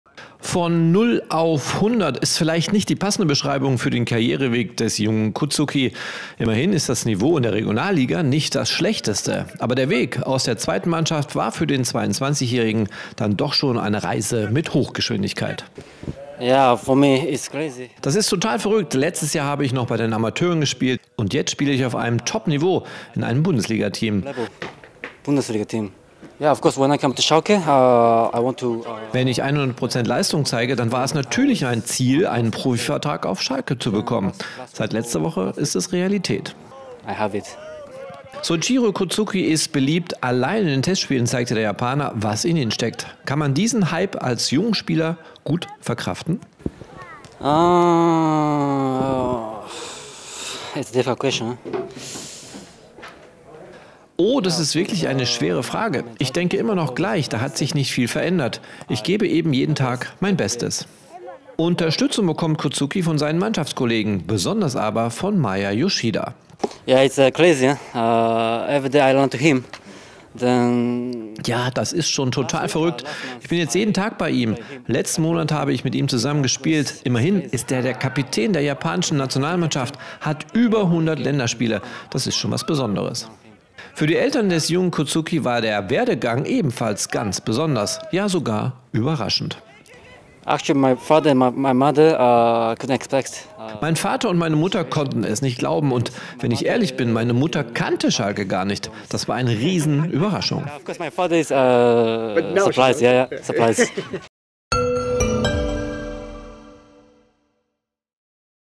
Von Null auf 100 in die Bundesliga: Soichiro Kozukis verrückte Zeit I S04
Von Null auf 100 ist vielleicht nicht die passende Beschreibung für den Karriereweg des jungen Kozuki. Immerhin ist das Niveau in der Regionalliga nicht das Schlechteste.
Aber der Weg aus der zweiten Mannschaft war für den 22-Jährigen dann doch schon eine Reise in Hochgeschwindigkeit.